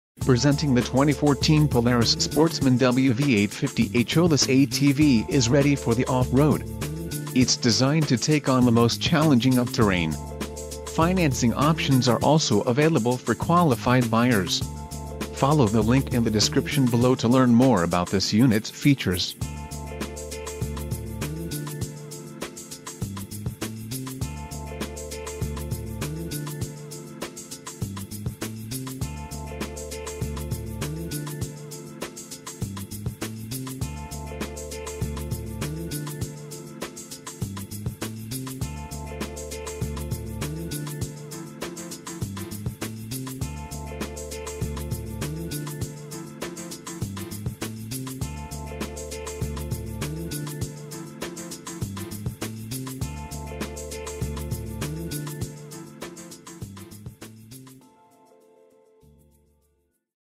2014 Polaris Sportsman® WV850 H.O. - Xtreme Machines - ...
Last One!    Never get a flat with revolutionary TerrainArmor non-pneumatic tires Built to work with upgraded shock package and 600 lbs. steel racks capacity Longer, less fatiguing work days with Electronic Power Steering (EPS) and unmatched 11.75 gal. fuel capacity Polaris powder-coated black steel cargo system with D-rings Fixed front winch Shift lever lock Full underbody skid plate Push bumper Wider foot wells Enhanced chassis Full body steel exoskeleton Engine Type: 4-Stroke SOHC Twin Cylinder Displacement: 850 cc Cylinders: Twin Engine Cooling: Liquid Fuel System: Electronic Fuel Injected Front Suspension: Dual A-Arm; heavy duty shocks Front Travel: 9 in. (22.9 cm) Front Brake Type: Single Lever 4-Wheel Hydraulic Discs with Hydraulic Rear Foot Brake Front Tires: 26 x 8-14 NPT Rear Travel: 10.25 in. (26 cm) Rear Suspension: Dual A-Arm, Rolled; heavy duty shoc... read more